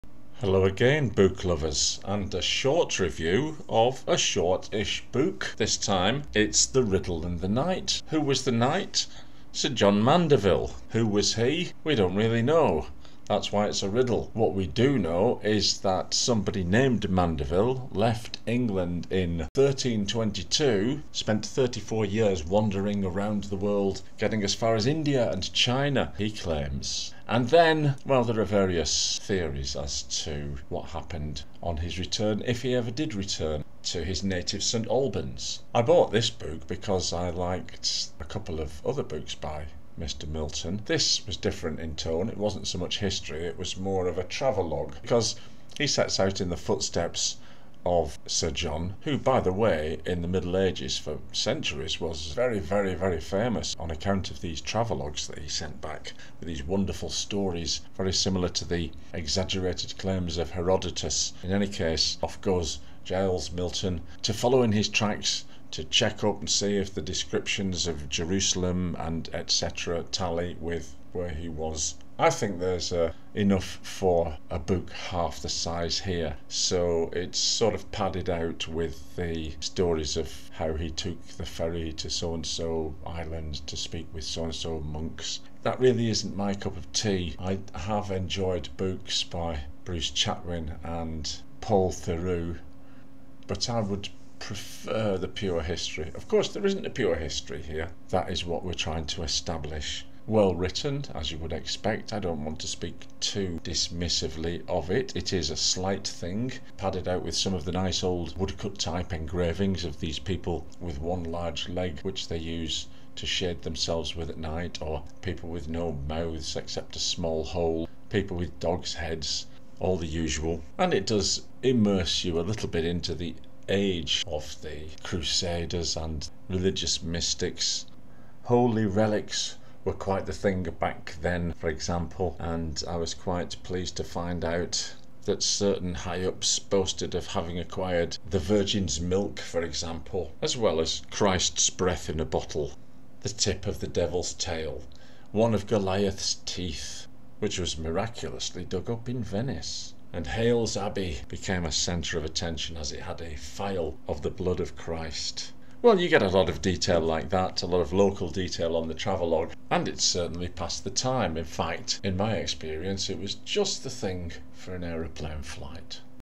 Book Review 36 The Riddle and The Knight by Giles Milton
Giles Milton follows a route that may not have been laid down in pursuit of a man who may not have existed. A nook or cranny of medieval history